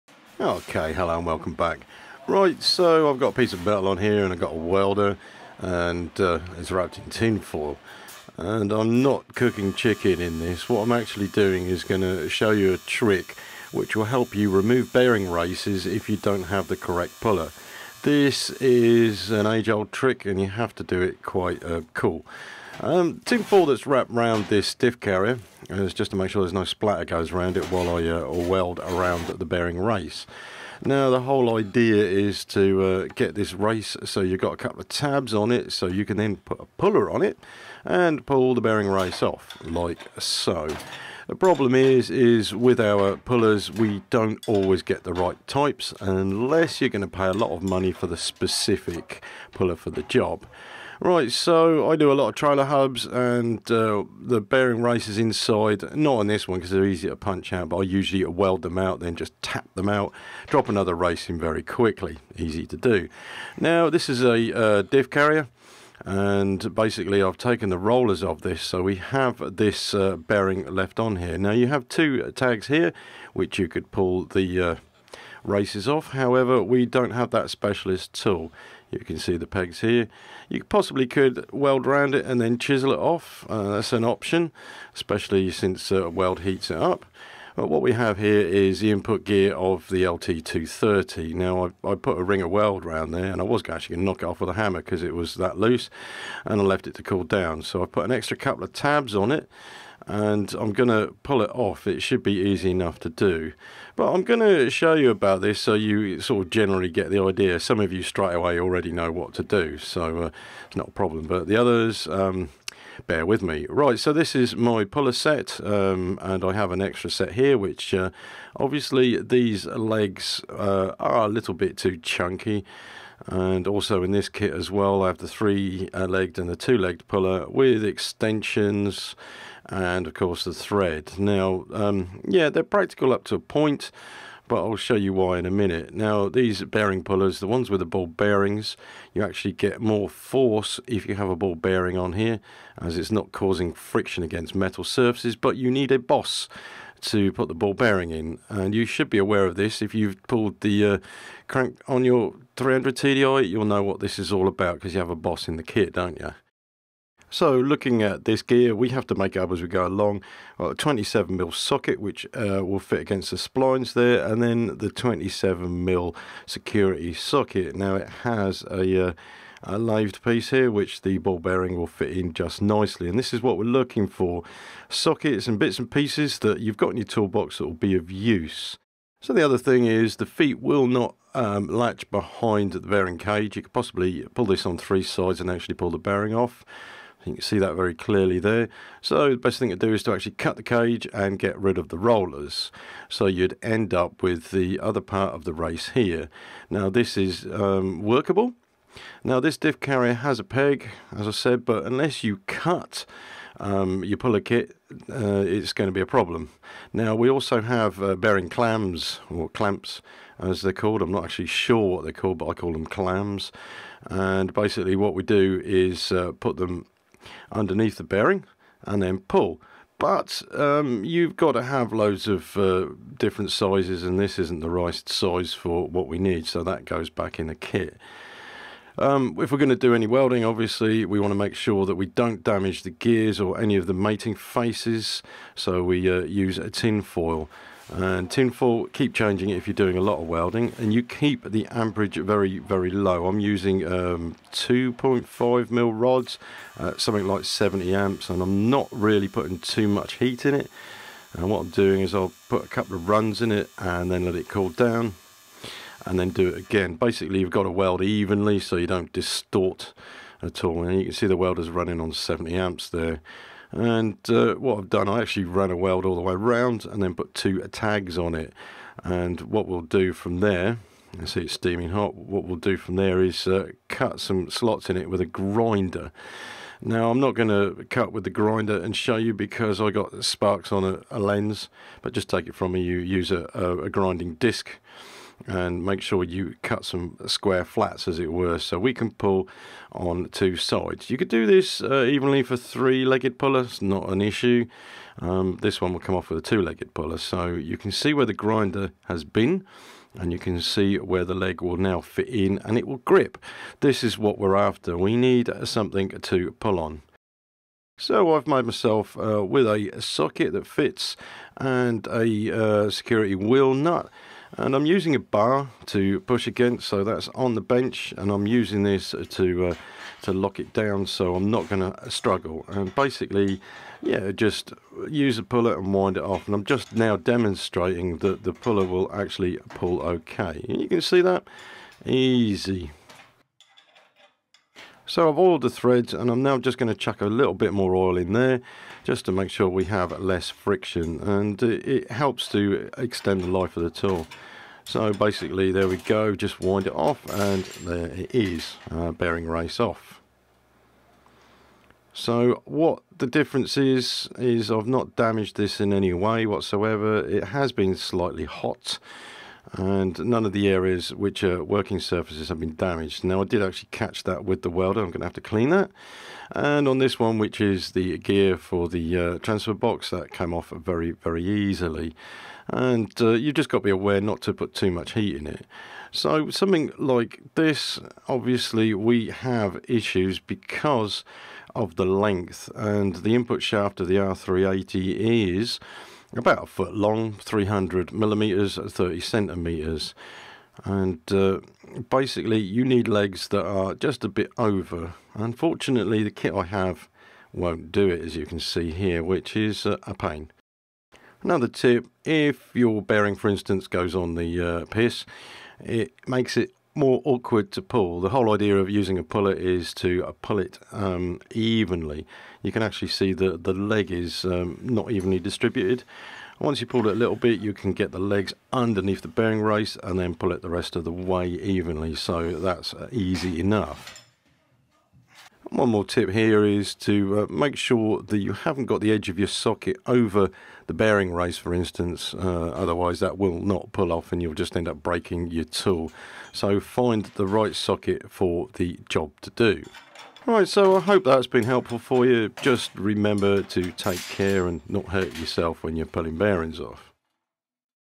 How to Effectively Pull a Bearing - when you don't have the right puller.
Just a tip that may help you in your hour of need.  Remember you may not have a welding machine but be assured, one of your Landy mates will.